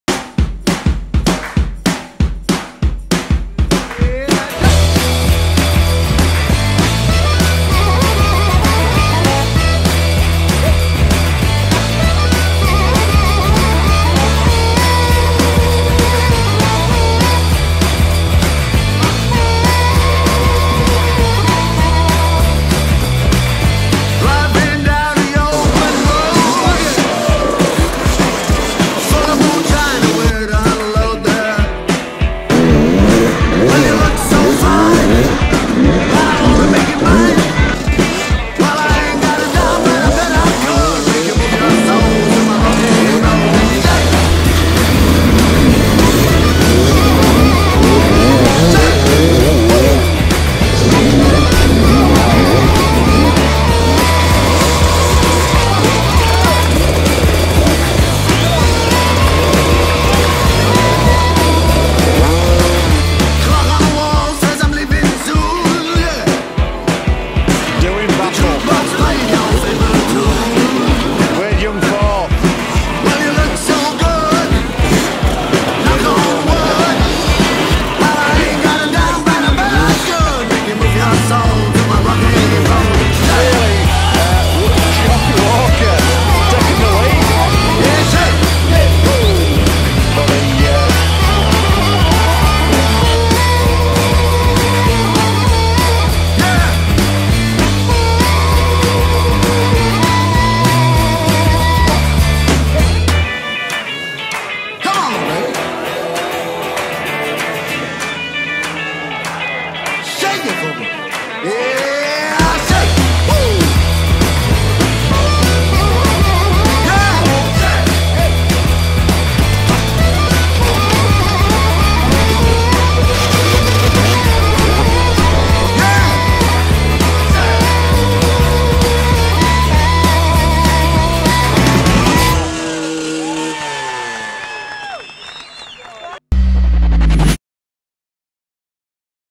EXTREME XL LAGARES 2017 || HARD ENDURO || PORTO PORTUGAL
Wade Young took the overall win at Extreme XL Lagares 2017. Early on in the race the leader changed from Alfredeo Gomez over to Mario Roman until finally at the first riverbed section Wade Young took the Lead. He then carried onto lead the remainder of the race.
The terrain was constantly changing from wet and slippery to dry and dusty. However Wade Young still managed to loose no time and stay ahead of the other pro riders. His techniques and overall riding skills were immaculate!

Jonny Walker claimed second place as he worked his way up in rank past his competitors. Overall Jonny did extremely well considering this was his first time attending Extreme XL Lagares.

Alfredo Gomez did not make it to winning his hat-trick as thought after he took first place on both the prologue and the night time enduro-cross. However he took third place after having issues with his clutch and having to stop in the pits for a short while. In this time his competitors continued and Jonny Walker moved up to third place.

The race overall was definitely unpredictable and exiting to watch. The crowds were amazing and so supportive towards the riders creating an energetic and vibrant atmosphere.